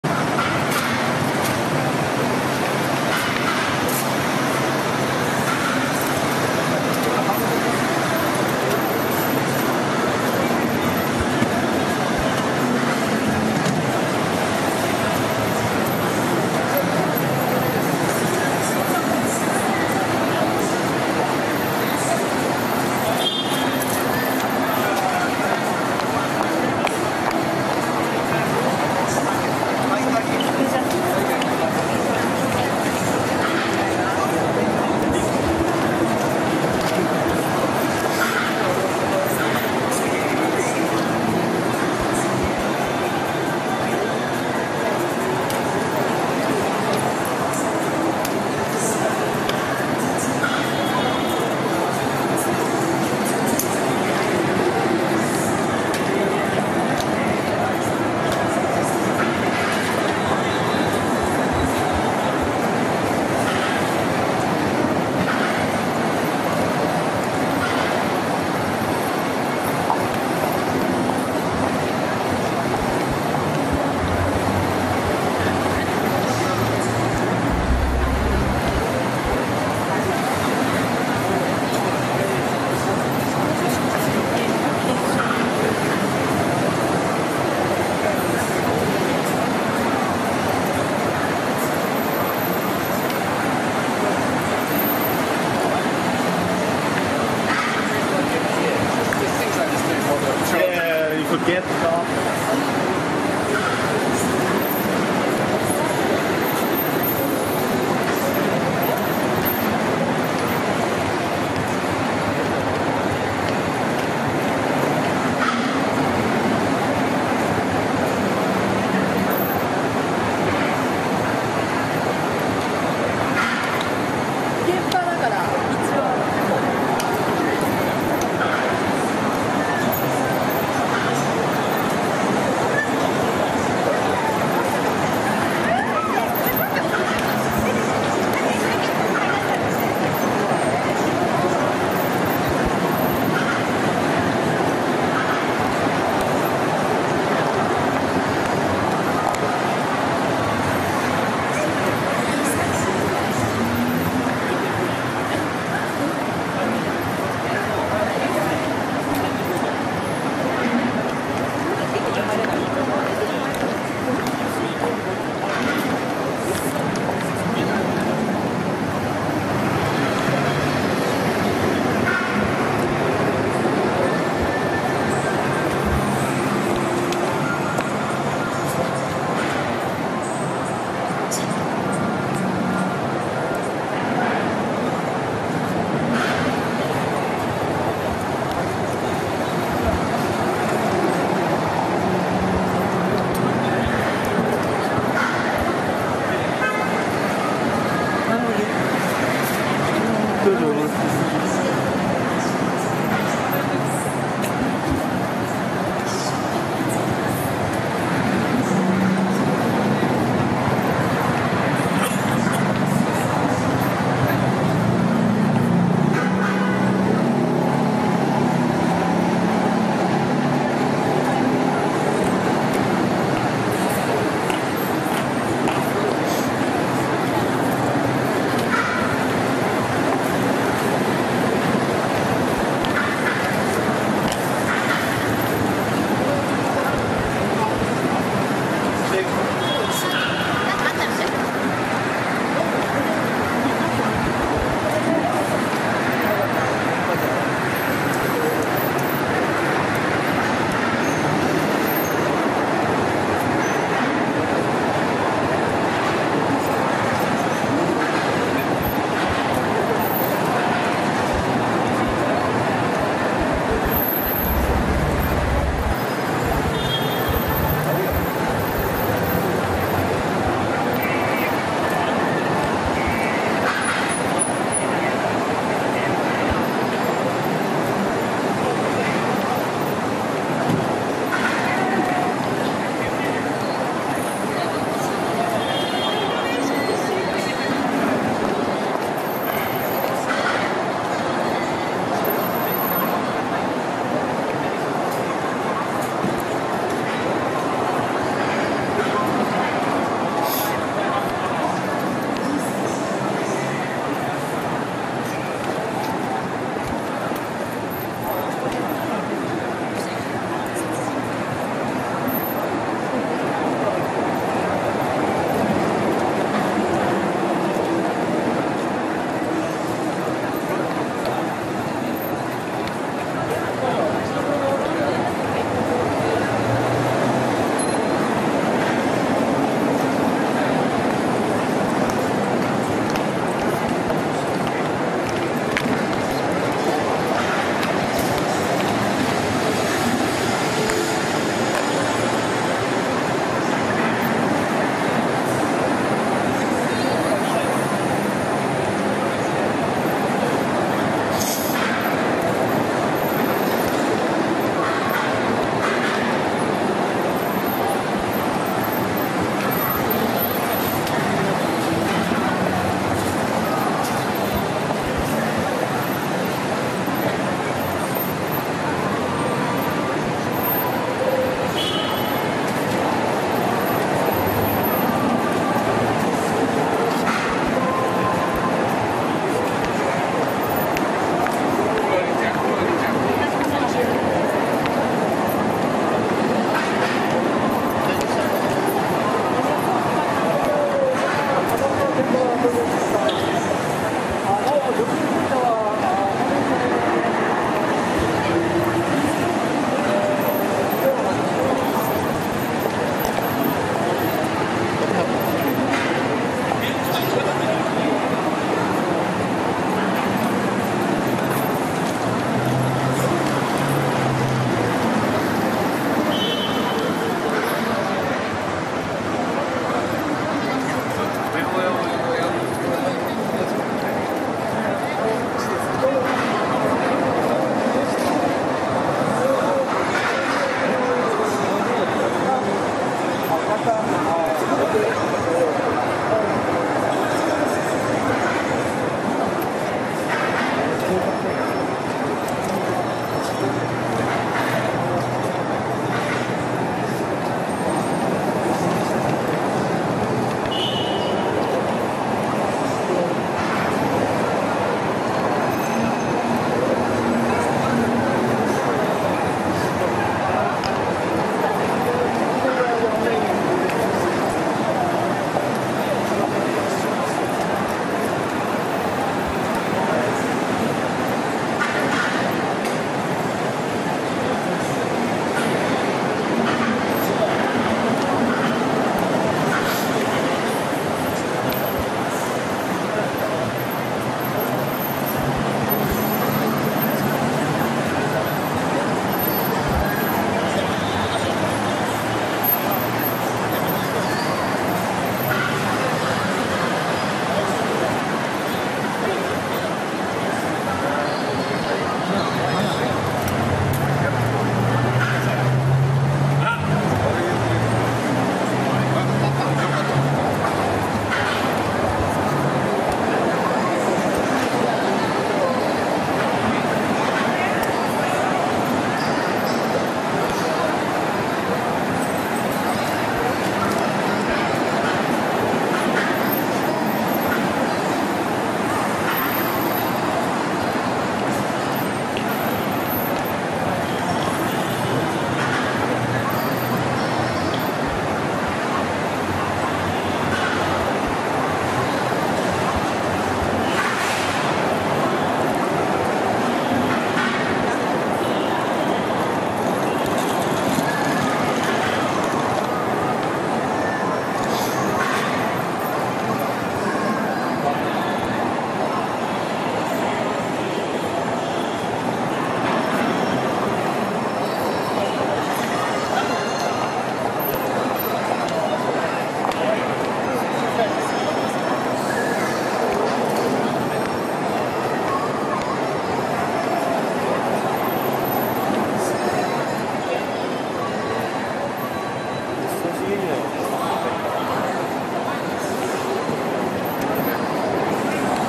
Death in Roppongi ????
I was walking down Roppongi dori when near Roppongi Crossing I noticed some emergency activity on the other side of the street.  Some dude with his neck all twisted was on the sidewalk.  After gawking for a few minutes, I pulled out the camera as they put him in the ambulance.  There were two small pools of blood on the sidewalk where he was.  The ambulance did not leave for more than 10 minutes after he was loaded - I think he was dead.